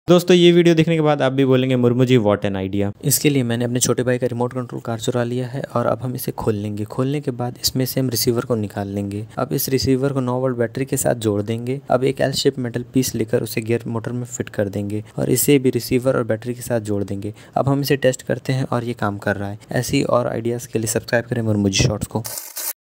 Remote Controller🎮 se banaya robot 🤖
Turn your remote control car into a robot it is awesome science projects with i made it in my school for science fair with help me to win 1st price

Steps to make a wireless switch bot robot :-
First take any remote control car then unscrew it from the bottom and there is a receiver and motor inside the remote control car now remove the receiver from the car and take 9 volt dc battery and attached it with the receiver and take a bicycle spoke and bend it in the L shape and fix it in the gear motor and attach it with the receiver and battery know attached it beside the switch and know you can operate your room light with the remote

Quarries:-
how to make a robot at home
remote control robot
diy robot
remote control car diy
remote control car experiment
how to make a robot